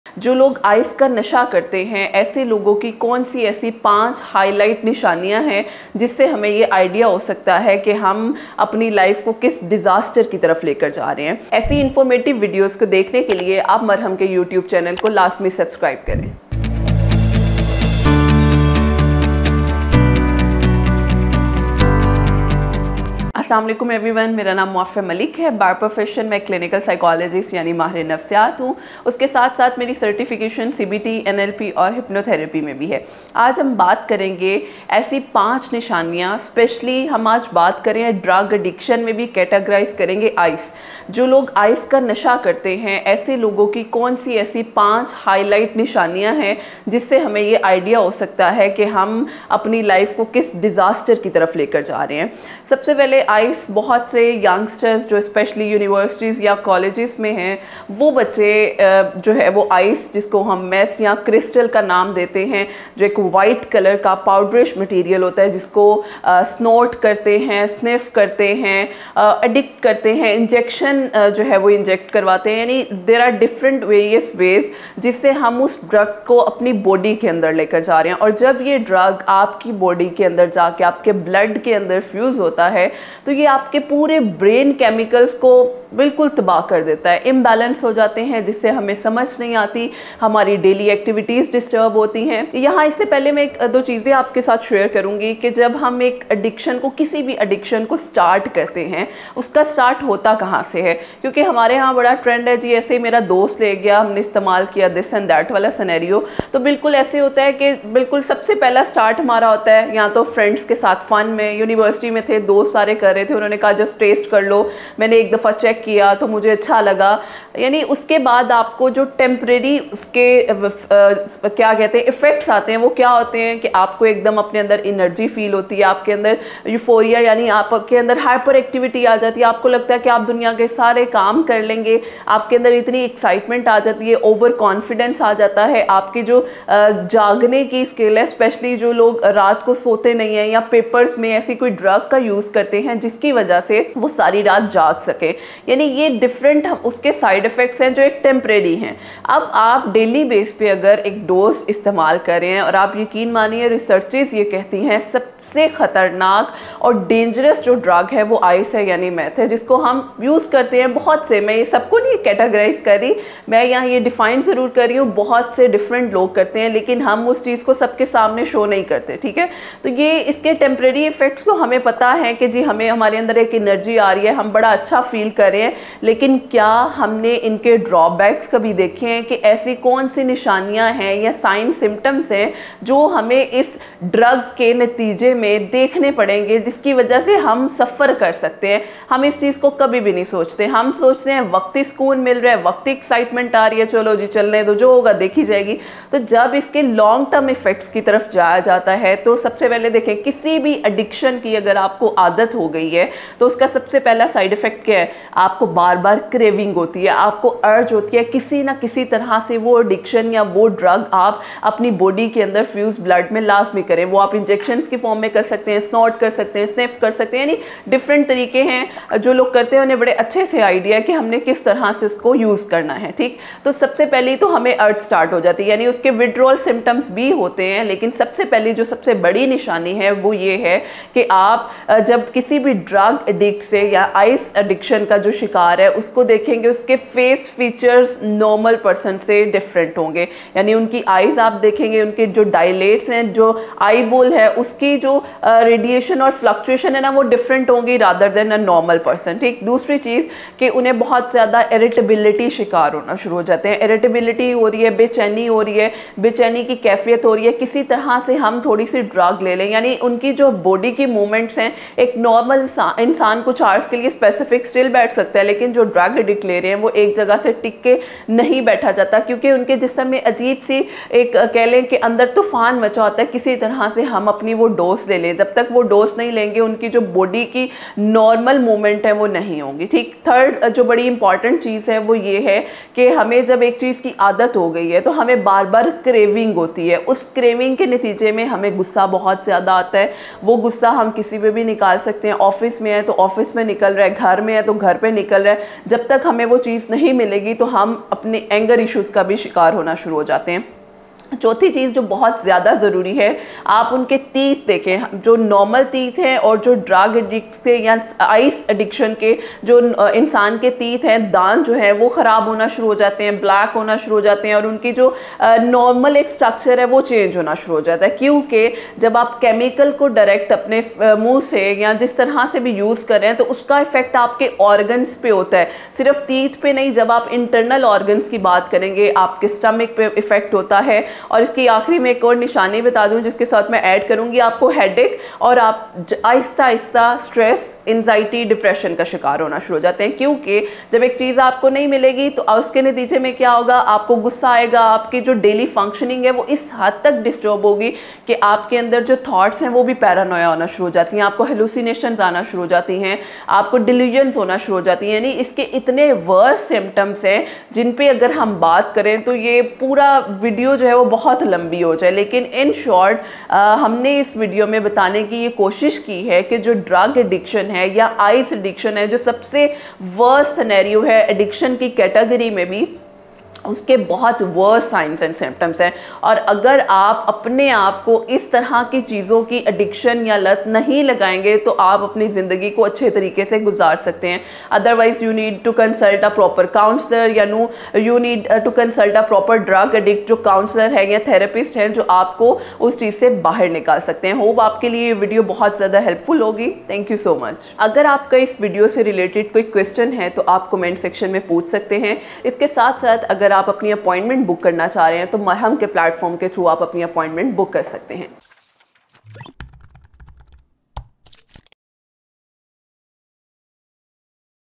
5 Ways To Identify Crystal Meth [ICE Drug] Addict | Drugs Complications & Treatment
5 Ways To Identify Crystal Meth [ICE Drug] Addict | Drugs Complications & Treatment

If you or someone you know is using crystal meth (ICE drug), know that there are risks associated with using this highly addictive drug.
 In this video, Ms. Moafia Malik will be discussing the signs and symptoms of crystal meth addiction and how to identify an addict. She'll also provide information on how to get help if you or someone you know is addicted to crystal meth (ICE drug).

Download the Marham App